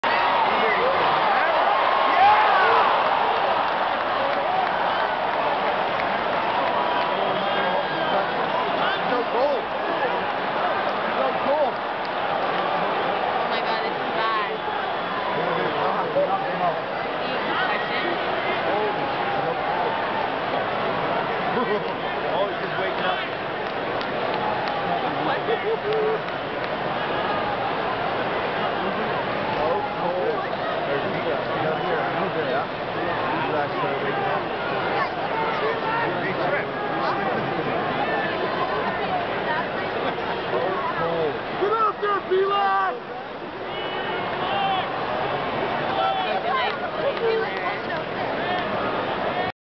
Newbury knocked out
Chris Newbury on the Leafs knocked out in a fight with some random NO NAME from the Pens. Feb10th in Toronto..